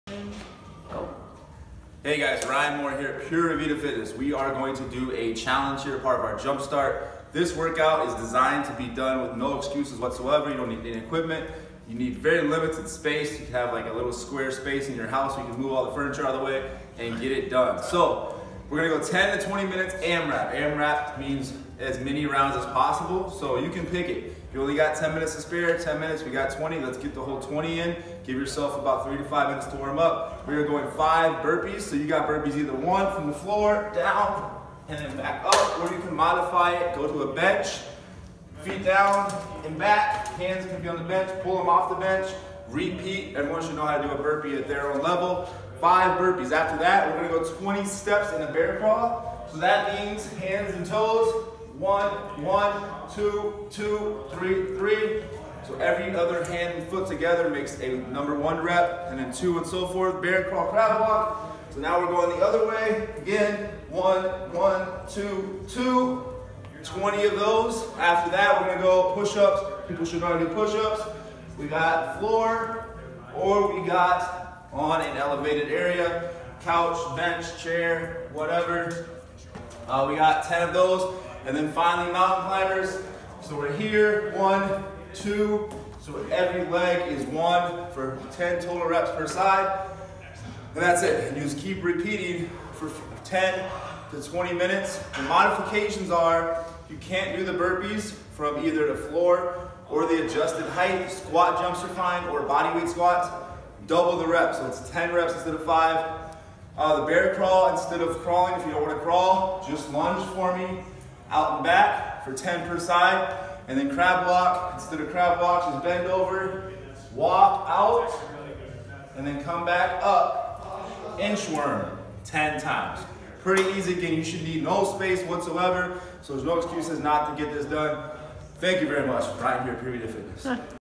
Lakeville Personal Trainer Jumpstart Self Fitness Challenge Week 2
This is a bodywight AMRAP
Burpees X5
Bear crawl X2 lengths
Crab walk X2 lengths
Push ups X10
Mountain climbers X10 each side